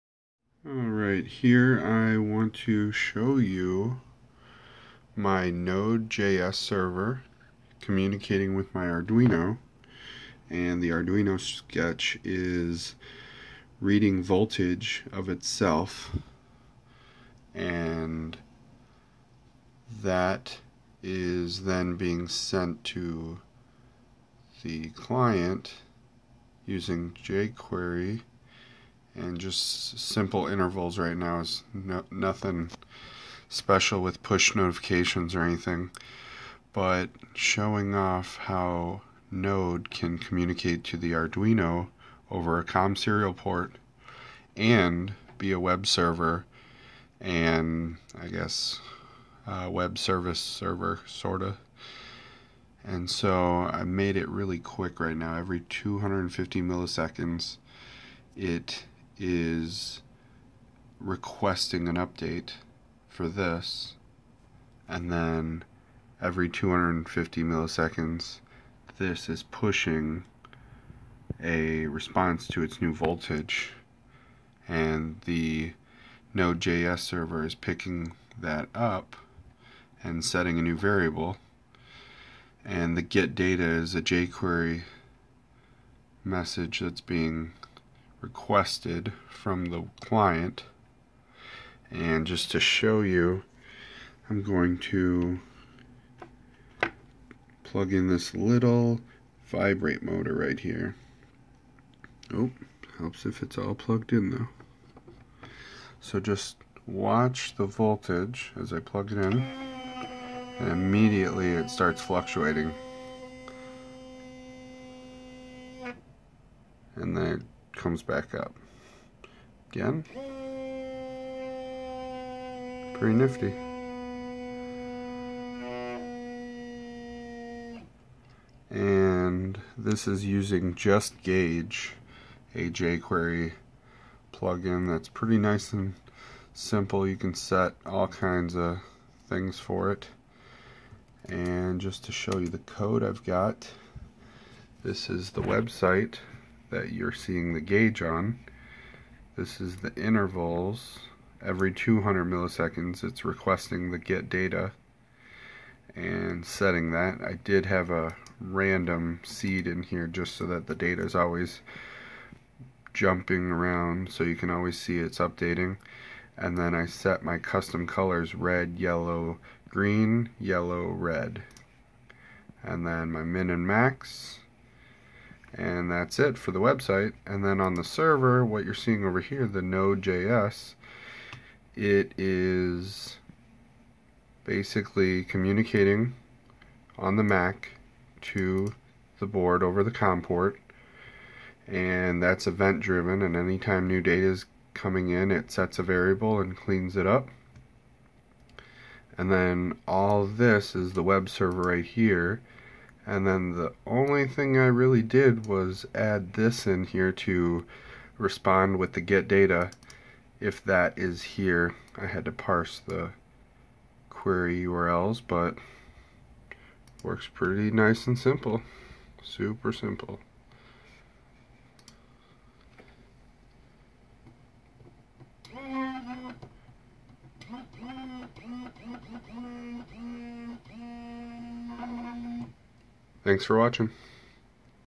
Arduino voltmeter nodejs COM webserver justgage
Showing off nodejs talking to an arduino on a mac using serialports and j query and justgage plug in. Let me know if u need anything.
Thanks